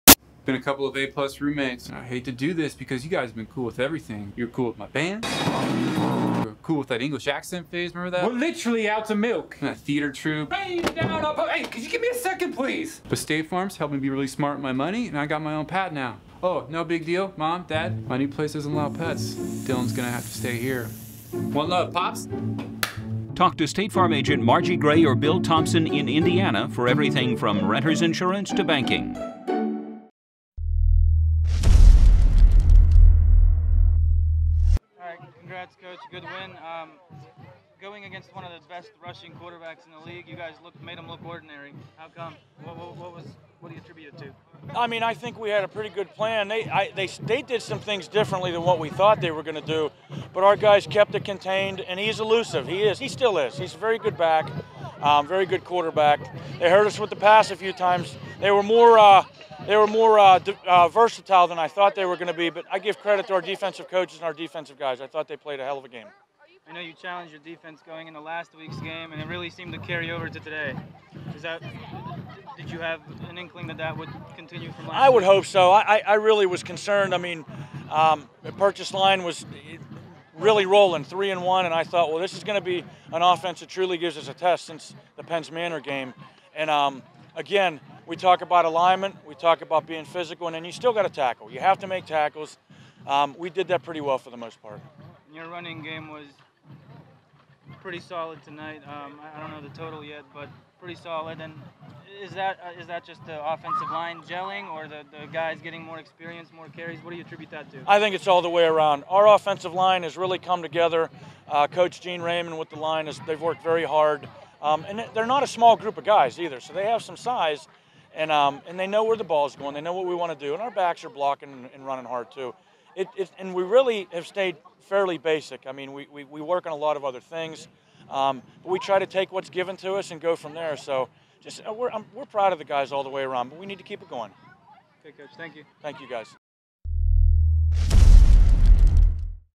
Post Game Interview with Homer-Center Coach Greg Page Homer-Center vs Purchase Line
Post Game Interview with Homer-Center Coach Greg Page
 Homer-Center vs Purchase Line
Mirza Zukic/ Indiana Gazette
James J. Nestor/ Indiana Gazette